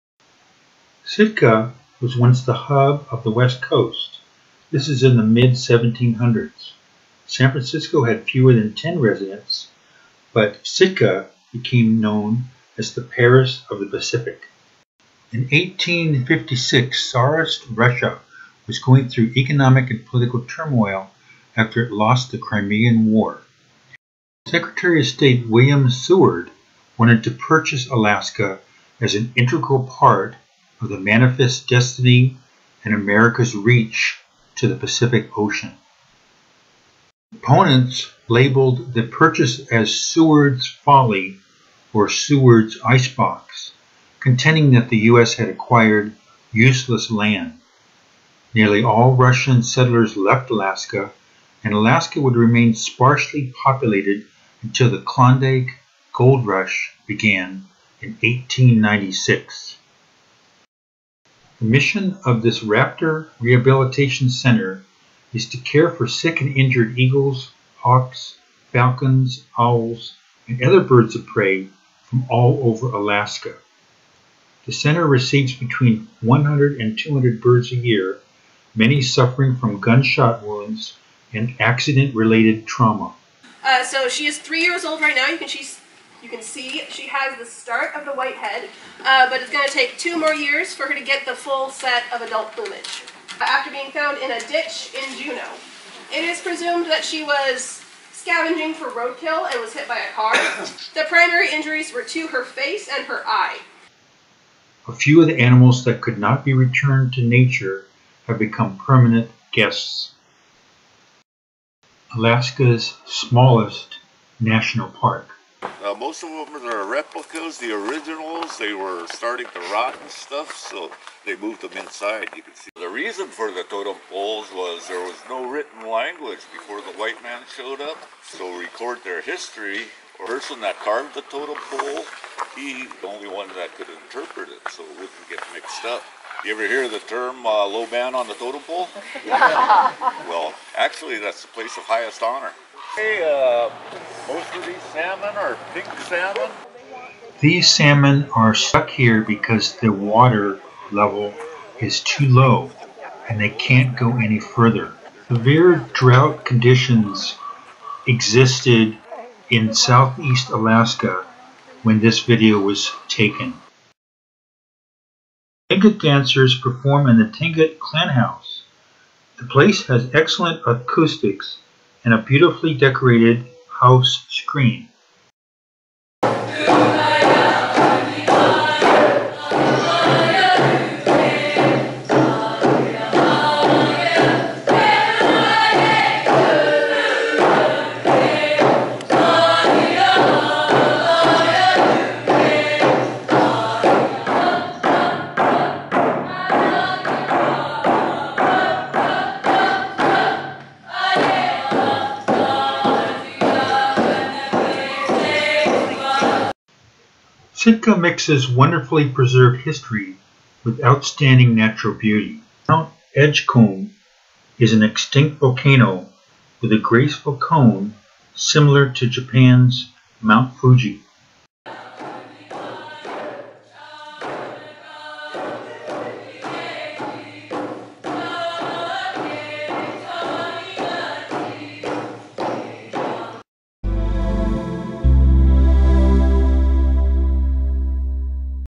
Sitka Alaska
Sitka, one of the oldest non-native settlements in Alaska and the former capital of Russian Alaska.

It is a mixture of well preserved history within an extremely beautiful setting and is only accessible by boat or plane as it is a series of islands in the Pacific Ocean. Vehicles are usually brought to Sitka via the Alaska Marine Highway ferry system.

However, a vehicle is not an absolute necessity in Sitka, as there are only 14 miles of road from one end of the island to another.